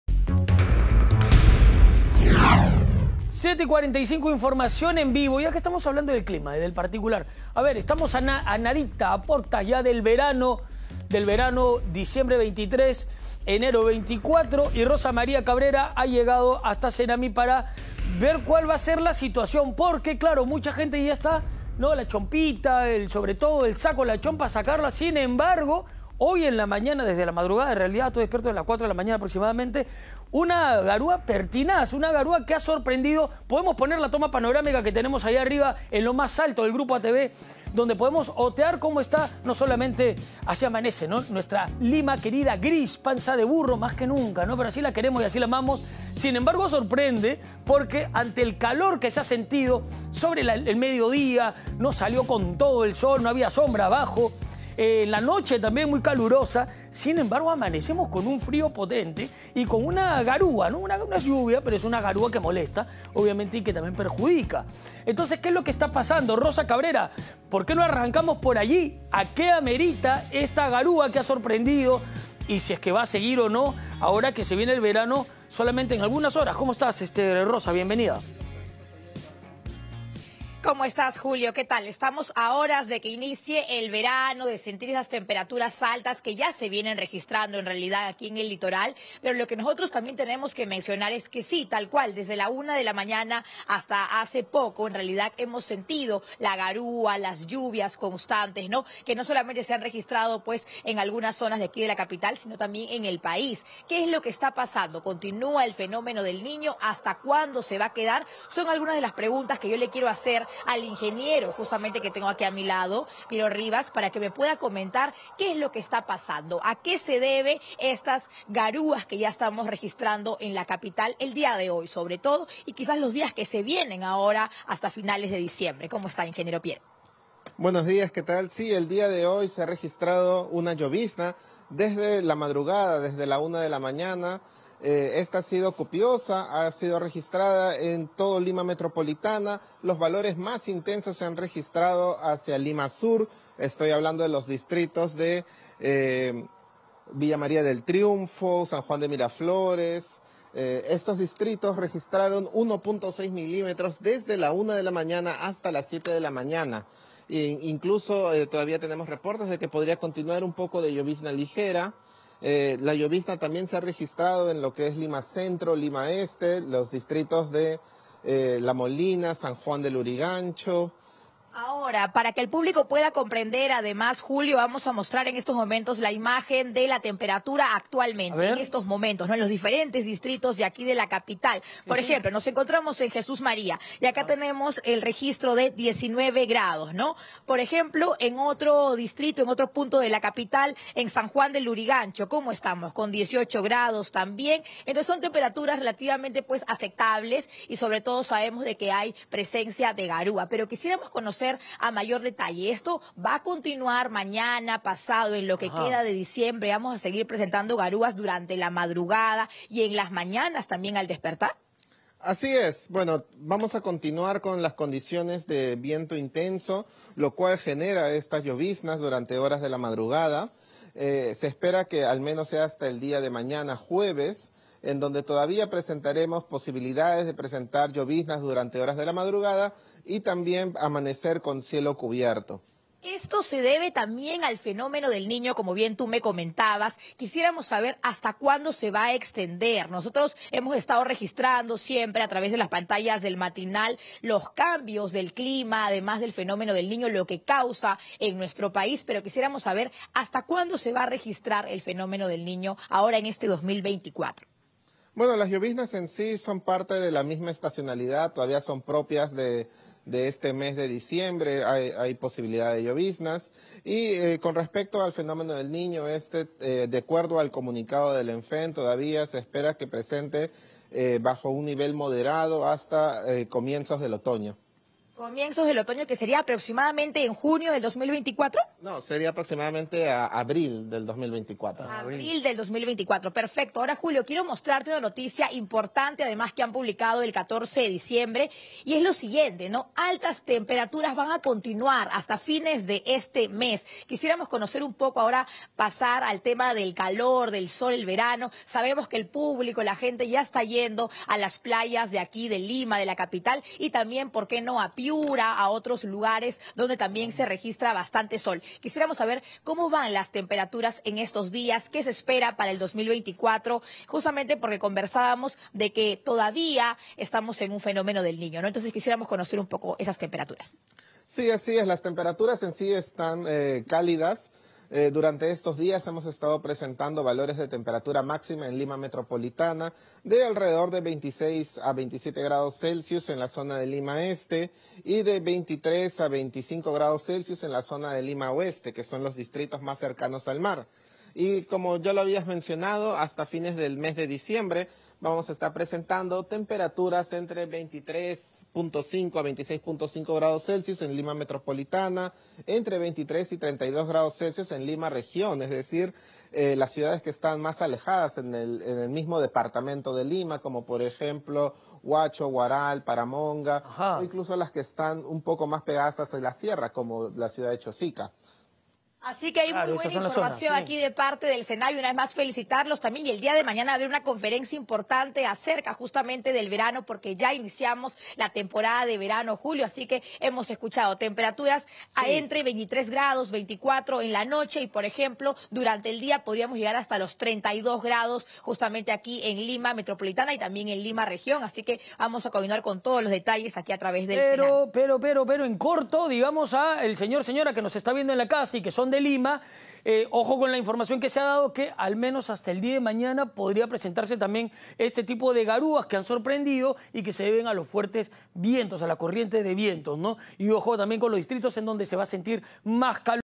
Lima amaneció con una fuerte garúa y Senamhi advierte cómo será el clima en los siguientes días
Conoce qué distritos de Lima serán los más afectados por las altas temperaturas del verano.